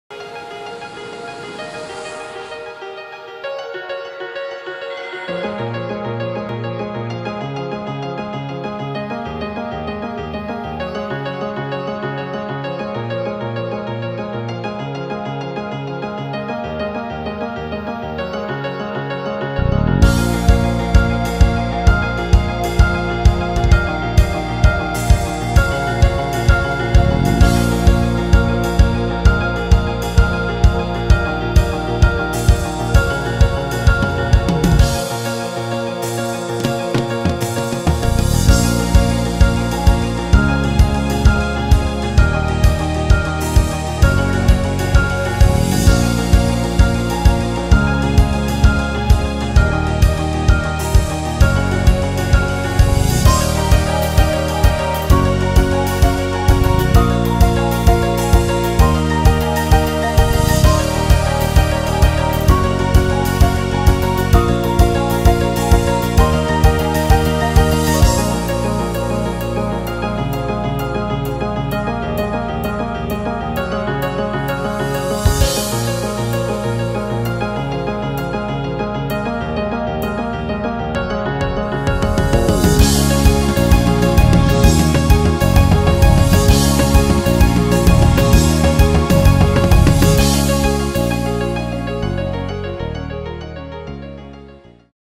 Teaser Essay Endurance 10H
Teaser fin de course Endurance de l'orne 10H sur le circuit International d'Aunay